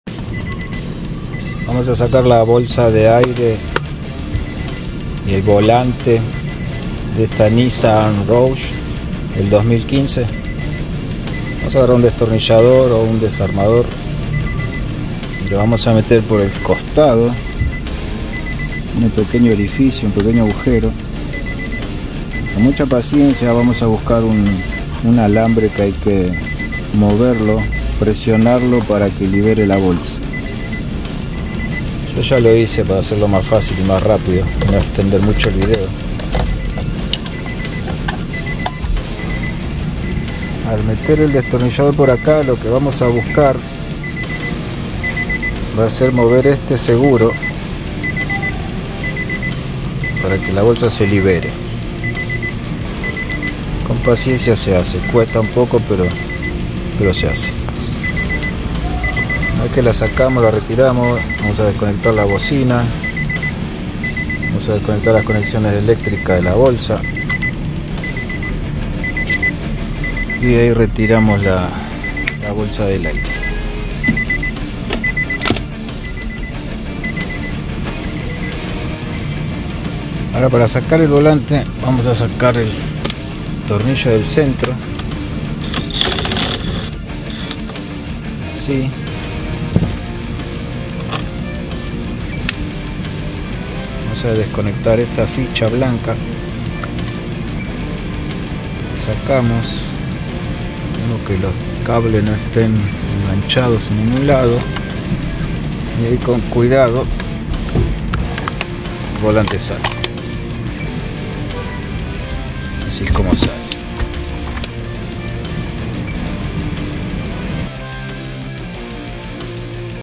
Nissan Rogue, sacar volante y bolsa de aire.
Como sacar volante y bolsa de aire de Nissan Rogue 2015.